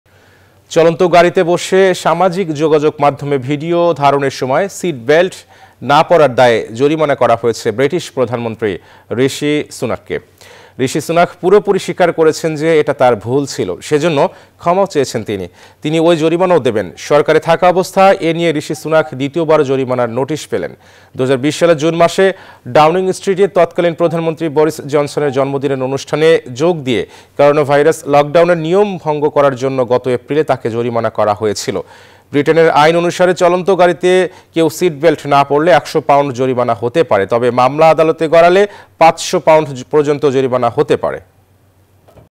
গাড়িতে সিটবেল্ট না পরায় জরিমানা ব্রিটিশ প্রধানমন্ত্রীর | News24
ঋষি সুনাক পুরোপুরি স্বীকার করেছেন যে, এটা তার ভুল ছিল। সেজন্য ক্ষমাও চেয়েছেন। তিনি ওই জরিমানাও দেবেন। সরকারে থাকা অবস্থায় এ নিয়ে ঋষি সুনাক দ্বিতীয়বার জরিমানার নোটিশ পেলেন।
Our other YouTube channels :
News24 Official Address:
NEWS24
371/A
Bashundhara Road
Block – D
Bashundhara Residential Area
Dhaka - 1229